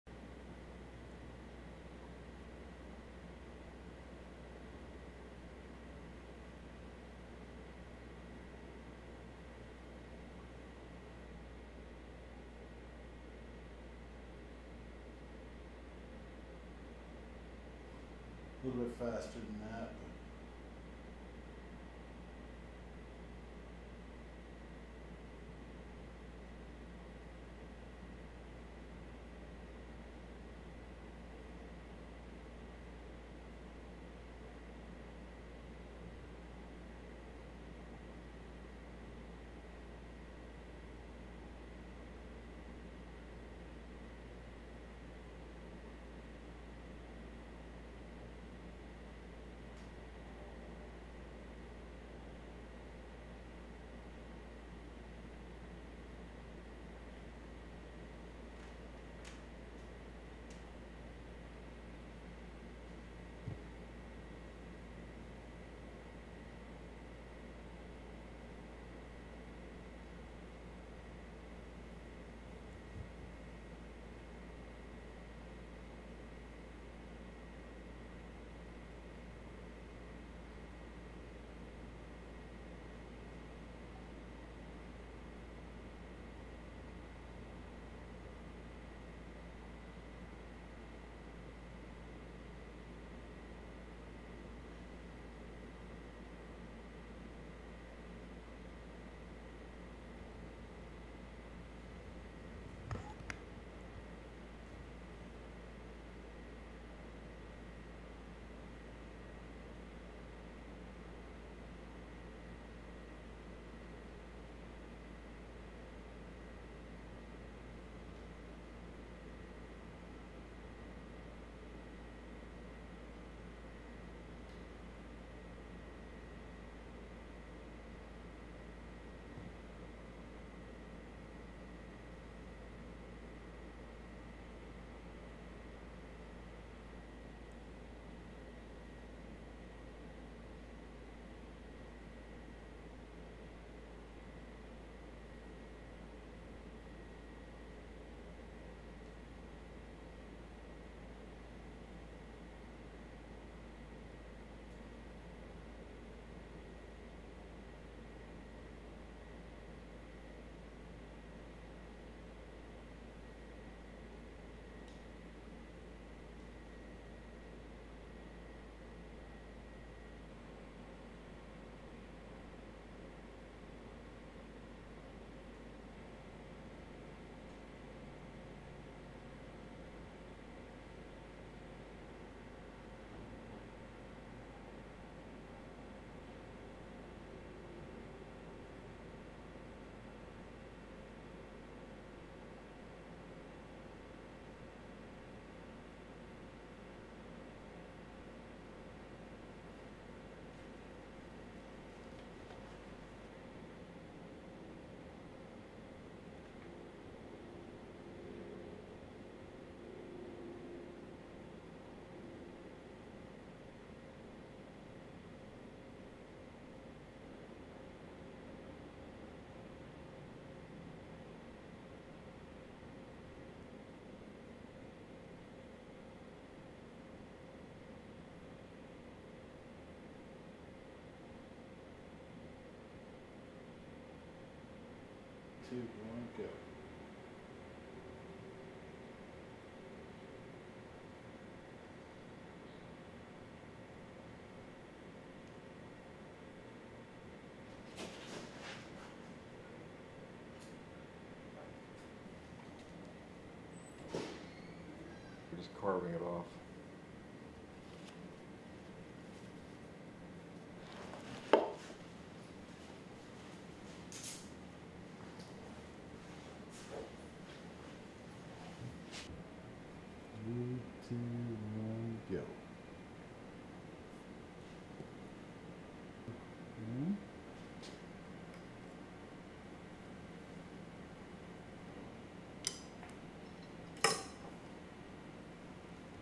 WEB EXTRA: The Creation of the CBS4 Nat Moore Trophy (Part 1)
Deran Wright is a sculpture artist who uses the traditional method of lost wax bronze to create his works. The Texas artist was commissioned by CBS4 to create the CBS4 Nat Moore Trophy.